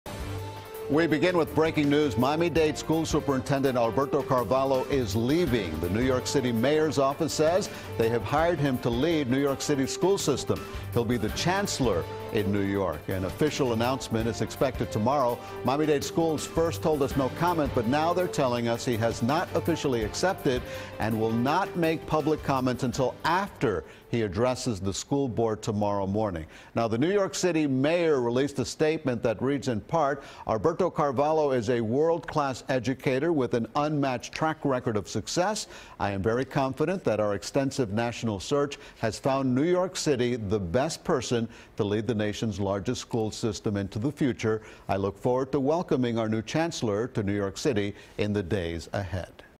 Miami-Dade Schools Superintendent Leaving His Post
CBS Miami's Eliott Rodriguez reports on Miami-Dade County Public School's Superintendent Alberto Carvalho taking a job in New York City.